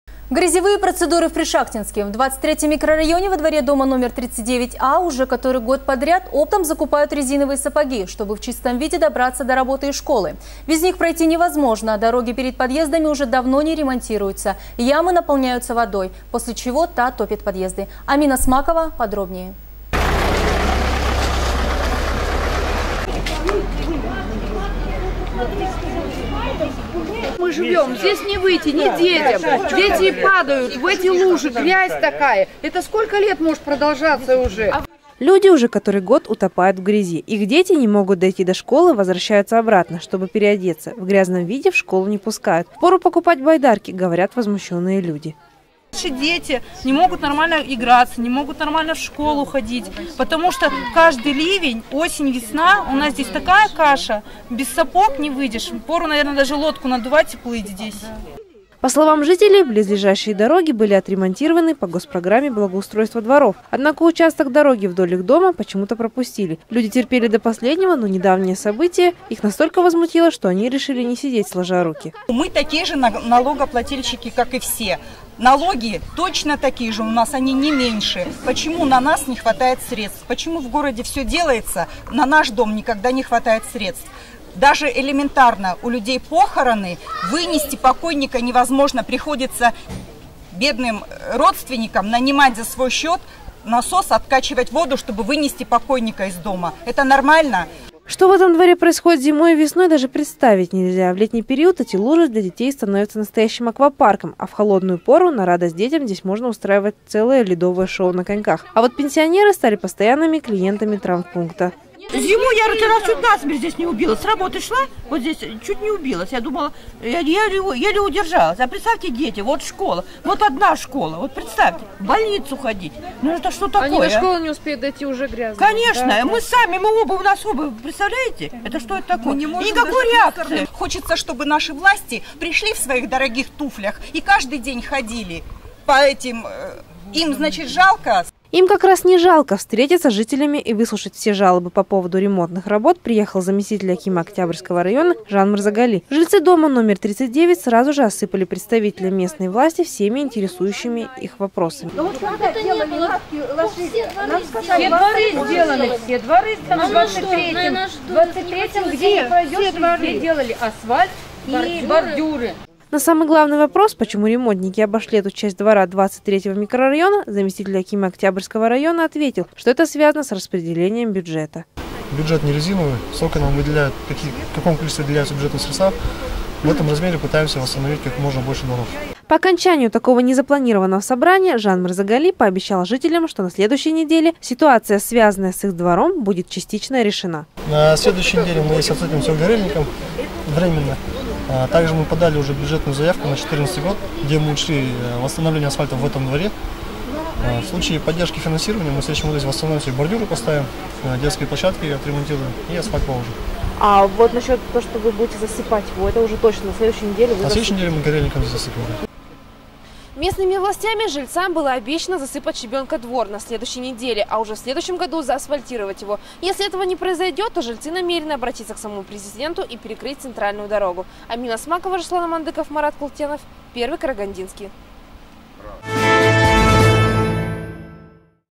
"Грязевые процедуры" в Пришахтинске.
В 23 микрорайоне во дворе дома № 39а уже который год подряд оптом закупают резиновые сапоги, чтобы в чистом виде добраться до работы и школы. Без них пройти невозможно, дороги перед подъездами уже давно не ремонтируются, ямы наполняются водой, после чего та топит подъезды.